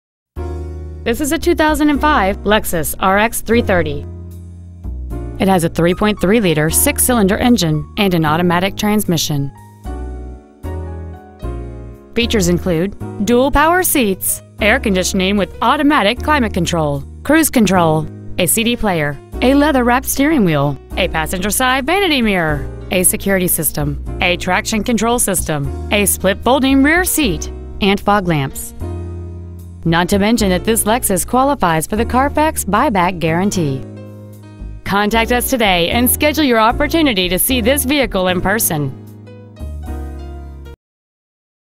2005 Lexus RX 330 Belmont CA 94002
Year : 2005
 Make : Lexus
 Model : RX 330
 Engine : 3.3L V-6 cyl
 Trans . : Automatic
 Exterior : Green
 Miles : 93,067
 Interior : Gray

 Preowned 2005 Lexus RX 330 Belmont CA